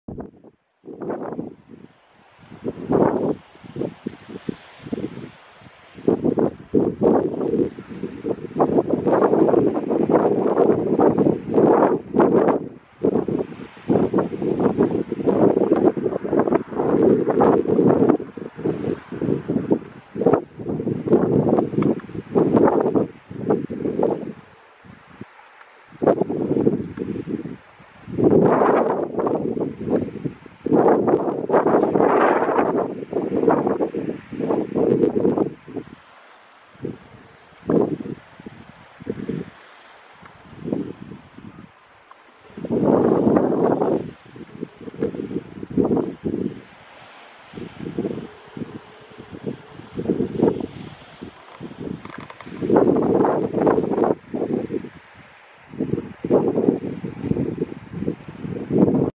Merbadise
Melmerby beach on a sunny spring day. April 2017